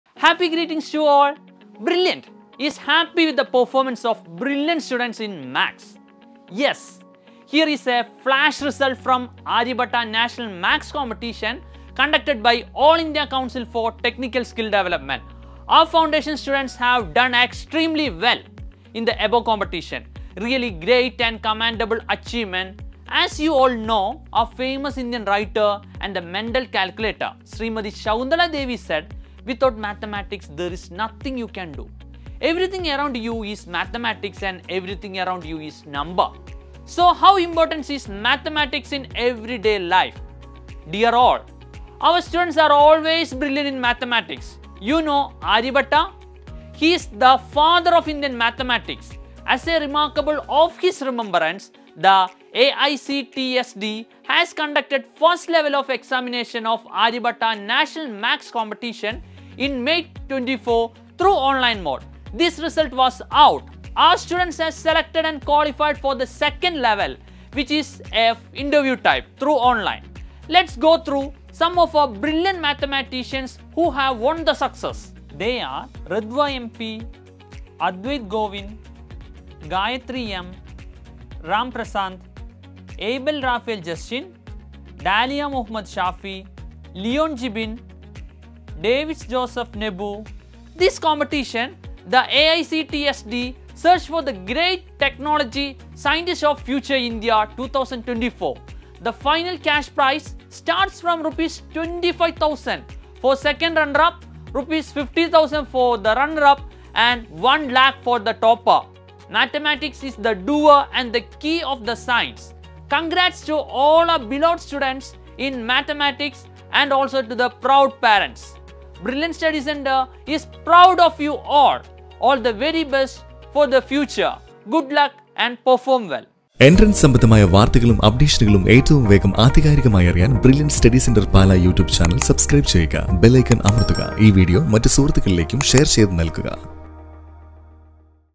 Brilliants in Maths | Aryabhatta National Maths Competition 2023-2024
Aryabhatta National Maths Competition is a prestigious competitive examination conducted by the All India Council for Technical Skill Development.  This exam aims to enhance the problem- solving ability and analytical skills of the young minds. In this video, we are presenting the toppers of this examination from Brilliant.